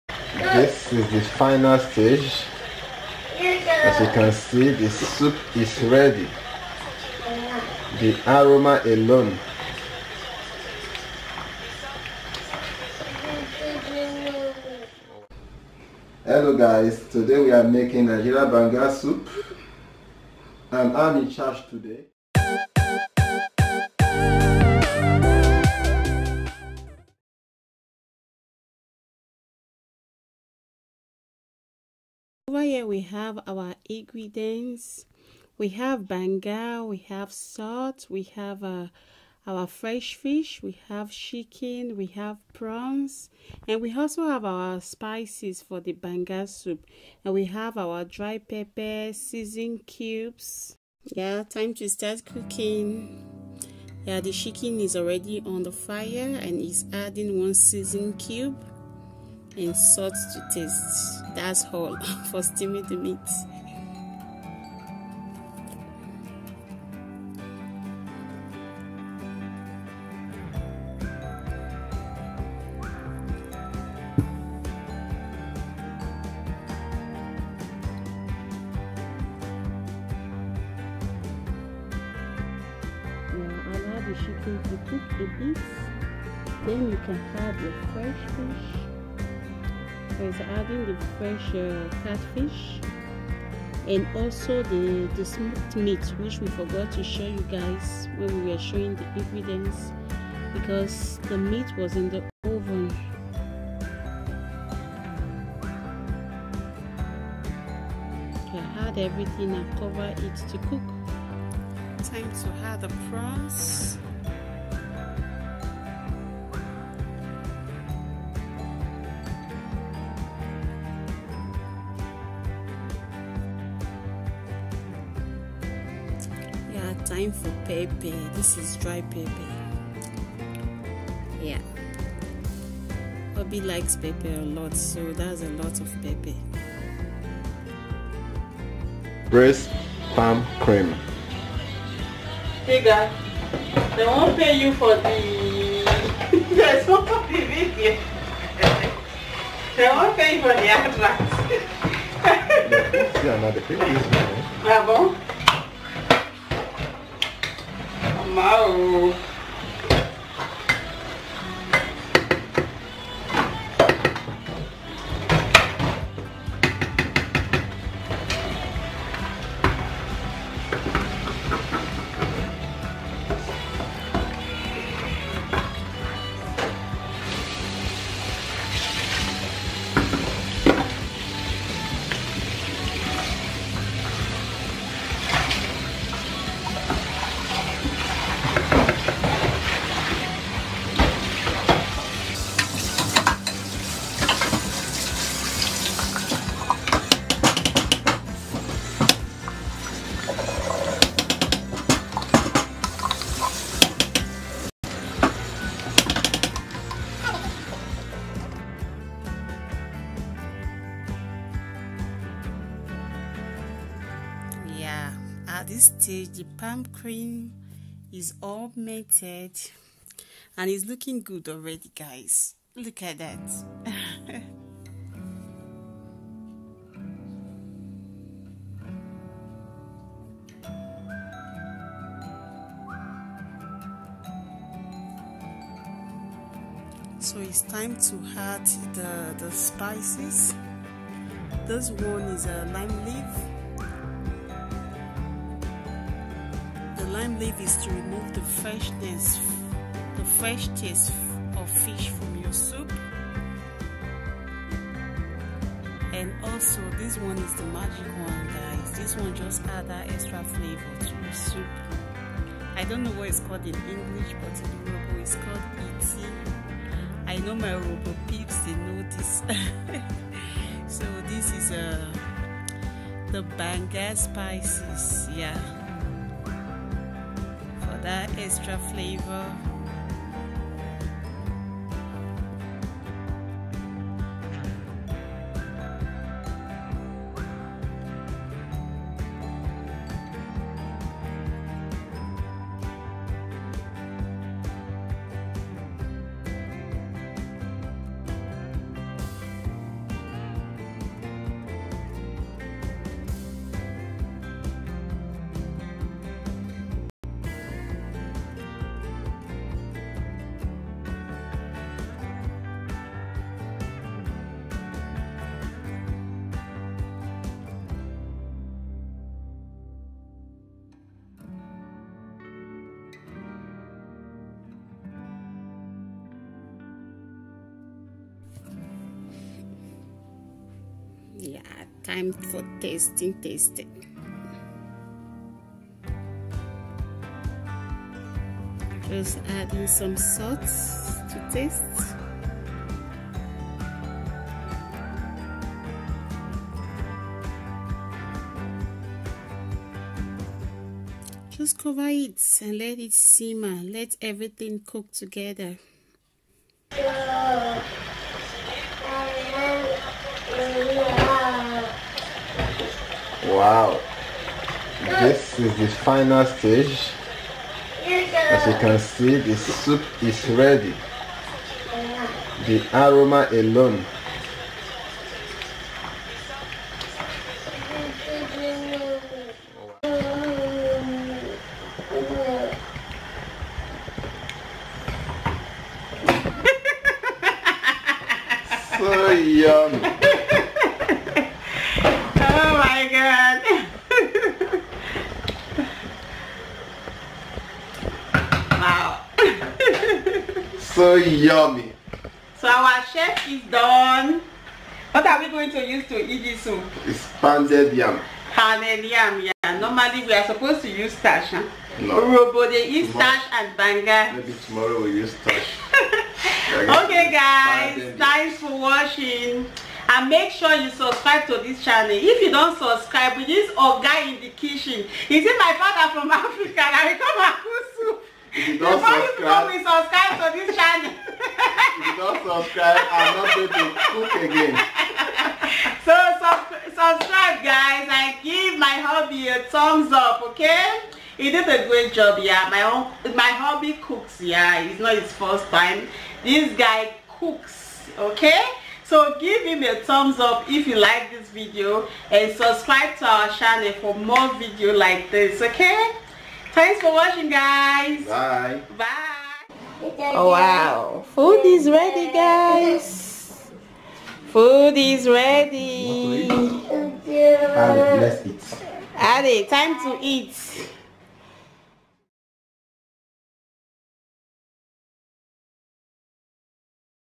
BANGA SOUP | My Hubby cooks the best BANGA SOUP 😍 |PALM NUT SOUP
Nigeriabangasoup urhobobangasoup palmcreamsoup bangasoup,